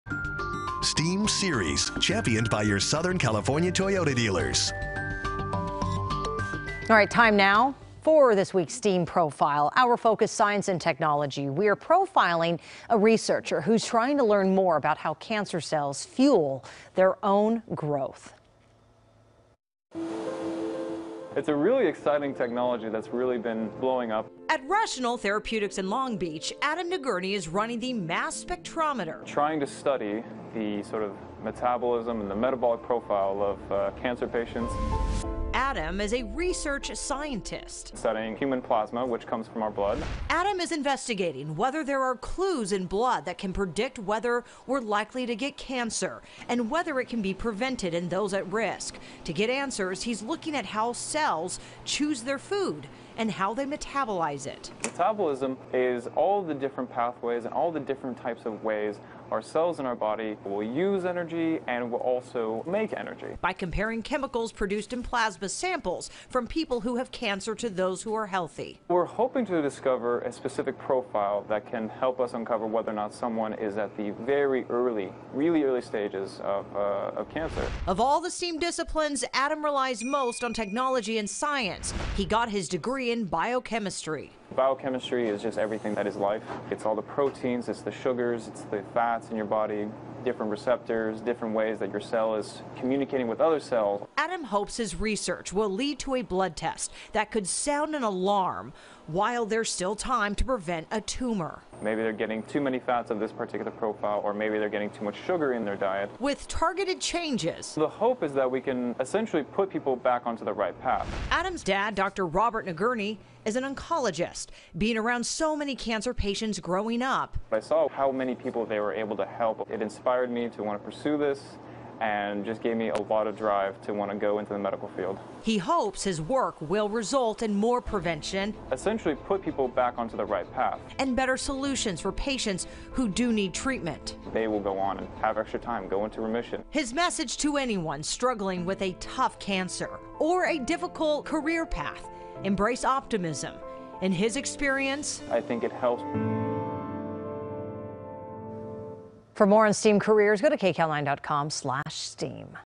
STEAM: Meet a cancer researcher
Adam Nagourney is studying the metabolic profile of cancer patients. Jasmine Viel reports.